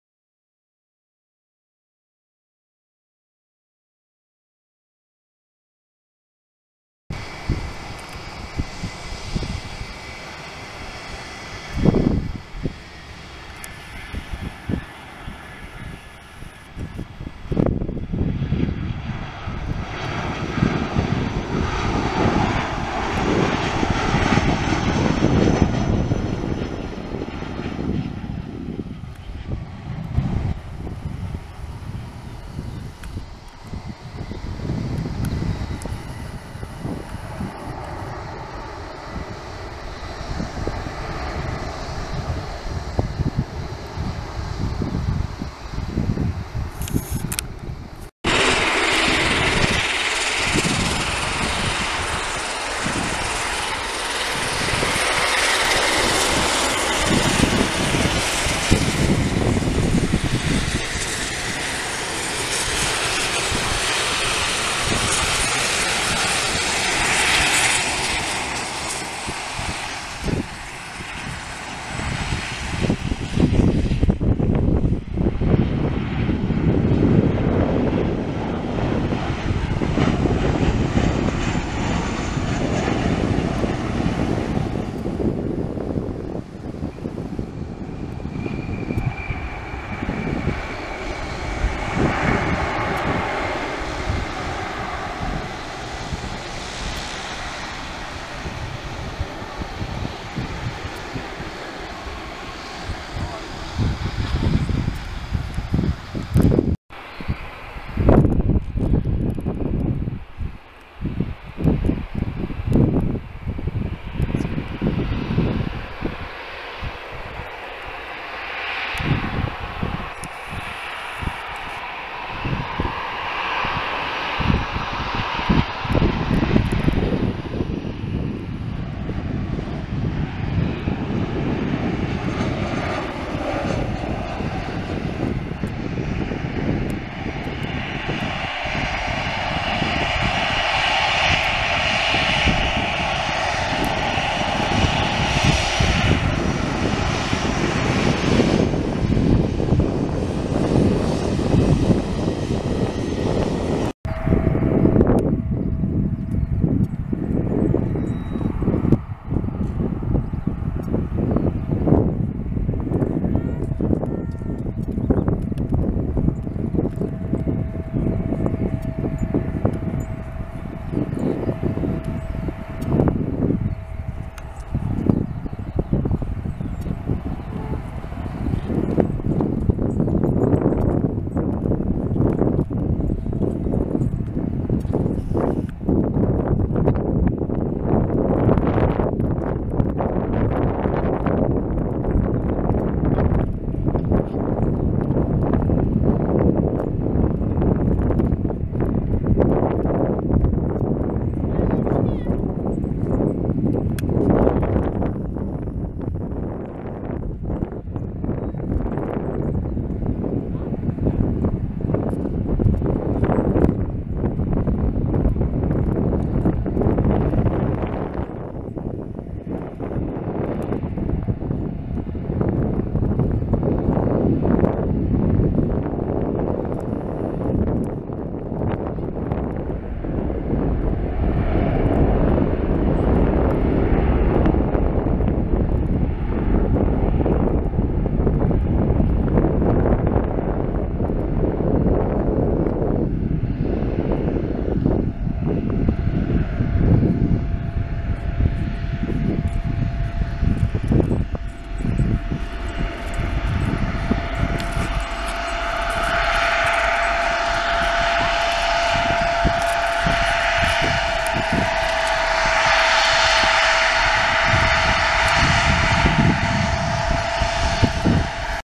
Stunning Edinburgh Airport 30/5/16
Great weather for some quick shots, including A320, Embraer E190 + 737's!! Watch the Ryanair!!